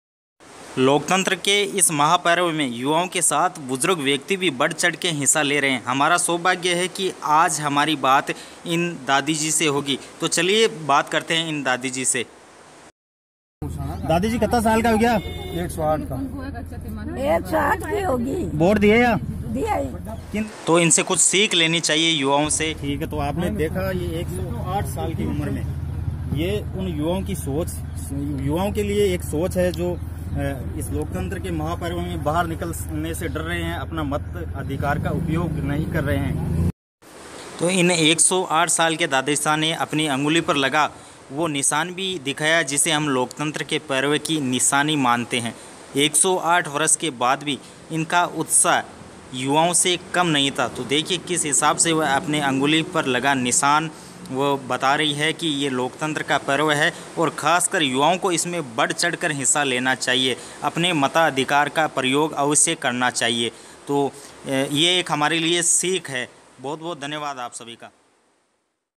108 साल की बुजुर्ग महिला ने डाला वोट | नागोर डीडवाना में क्या कहा 108 साल की बुजुर्ग दादीजी ने
In video
Loksabha election news
Nagaur Loksabha Election didwana me dala 108 saal kr bujurg ne vot